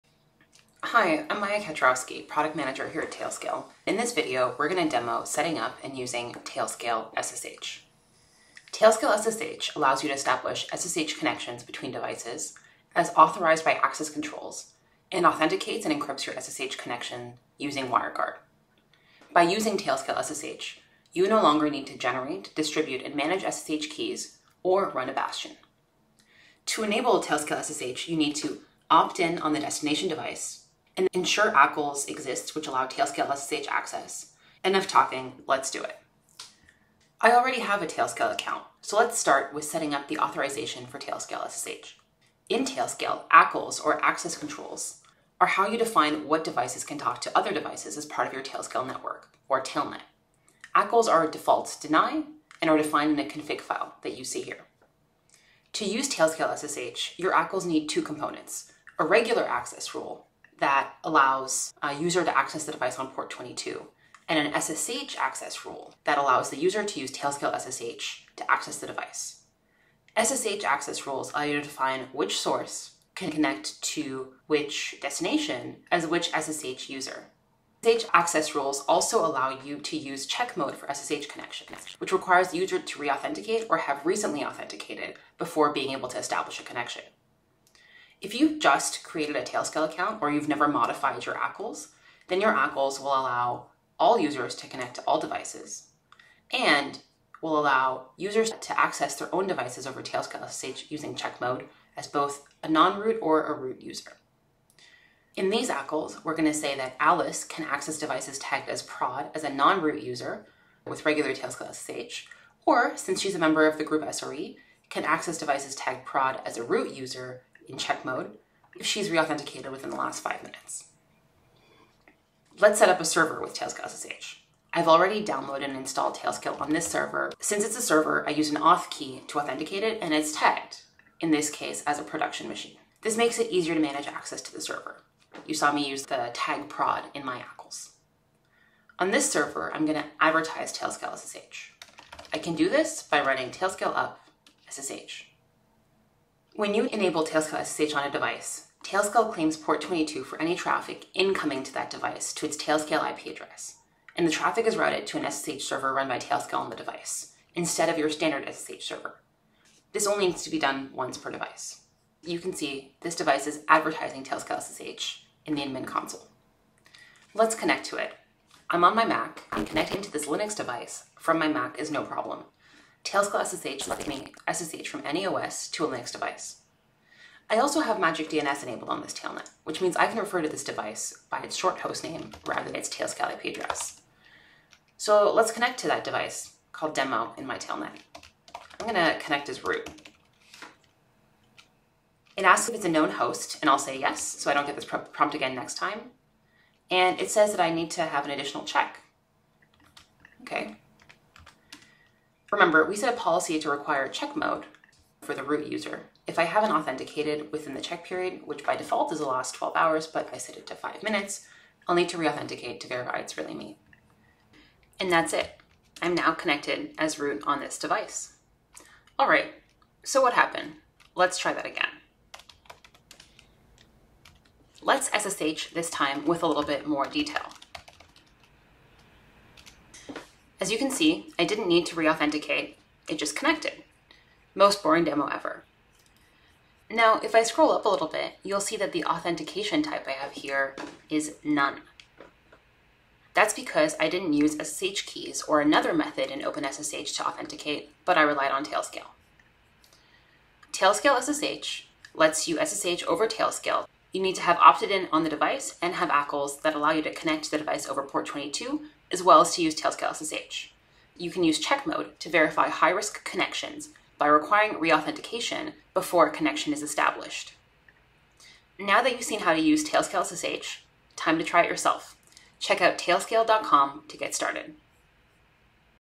Tailscale SSH Demo (full)
Our product manager, Maya Kaczorowski walks through a demo of Tailscale SSH in beta.